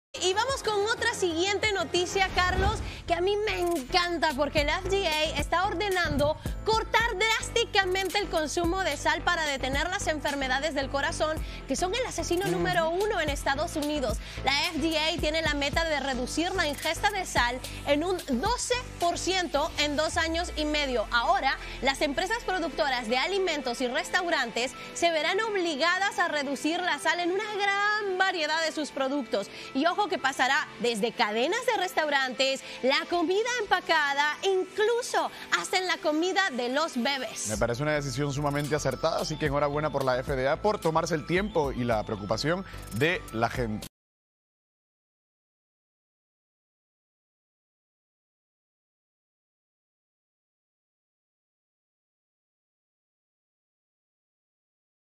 FDA ordena reducción de consumo de sal en alimentos para prevenir enfermedades | Telemundo
Con el fin de prevenir las enfermedades cardiacas, la FDA a ordenado la reducción de consumo de sal en los alimentos preparados.